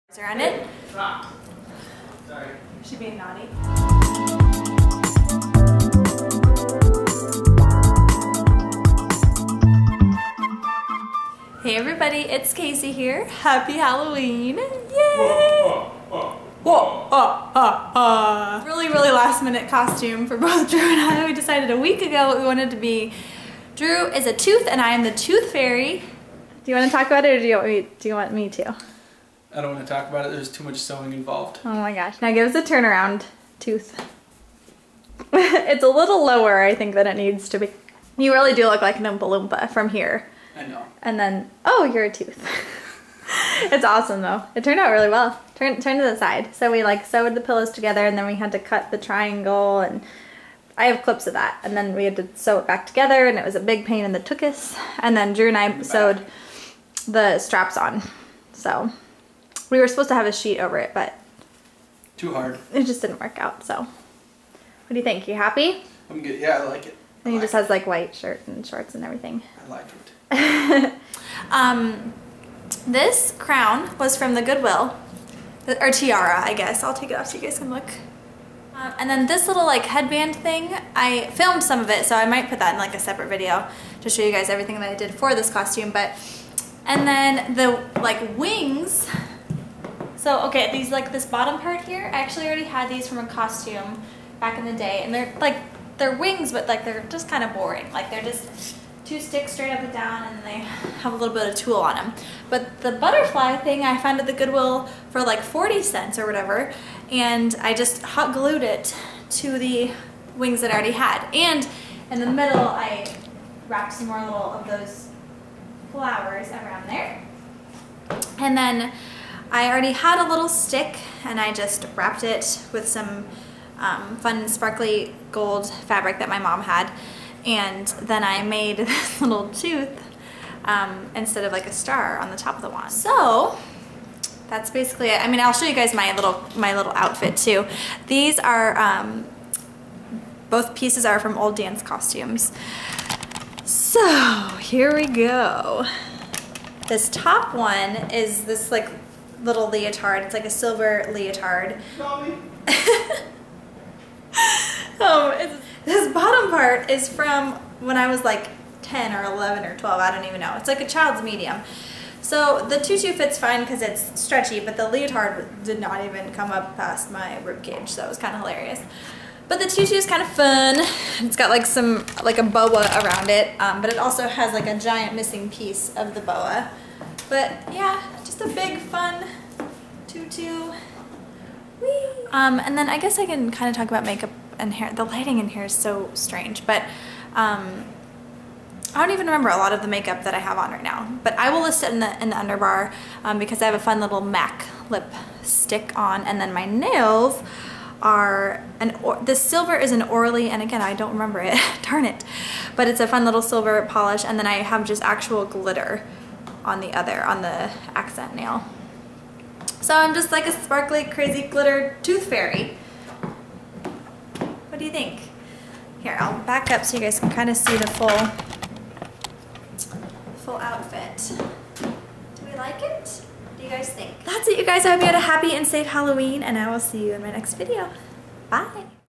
TOOTH & TOOTH FAIRY HALLOWEEN COSTUME!
Hope all of you had a happy and safe Halloween!!! I will be posting more detailed videos about the process of making/creating each of our costumes later this week. :) Let me know what you dressed up as! Love you all!!!

(I still need to fill in the specifics about makeup and nails):
Eyeshadow: Urban Decay NAKED palette (the original)
Eyeliner: E.L.F. black liquid liner
Mascara: Maybelline Falsies and Two Faced Better Than Sex
Lipstick:  by Mac
Nails:  by Orly